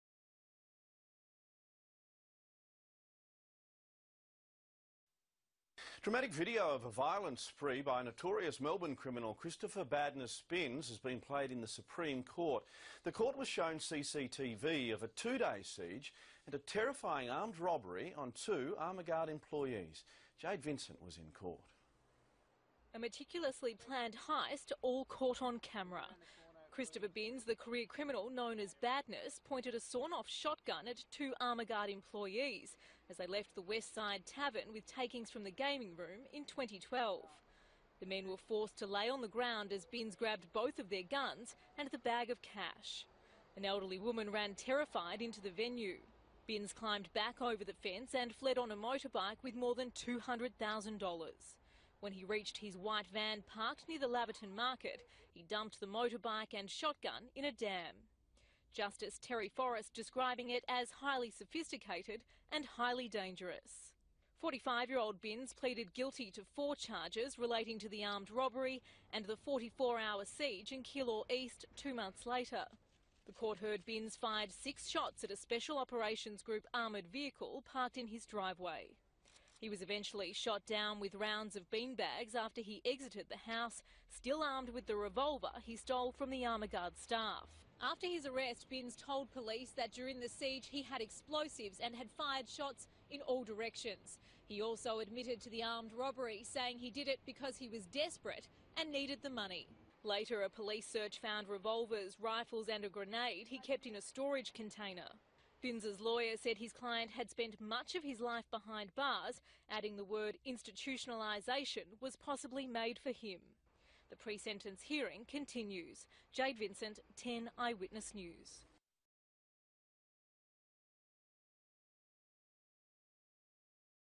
CCTV of Christopher "Badness" Binse robbing guards at gunpoint
A man who was at the centre of a two-day siege in Melbourne's north-west in 2012 has been sentenced to more than 18 years in prison.

Christopher Dean Binse, who goes by the nickname "Badness", shot at police multiple times during the 44-hour siege at his Keilor East home in May 2012.

Police fired tear gas and beanbag bullets at Binse before they arrested him, and charged him with several firearms and robbery offences pertaining to the siege and an armed robbery in March 2012.

The court heard Binse ambushed two Armaguard employees with a shotgun outside a Laverton hotel and stole more than $230,000 in cash and two revolvers.

Binse spent more than 28 years of his life in custody after his father taught him to steal.

He spent much of that time in isolation in the notorious Acacia Unit at Barwon Prison.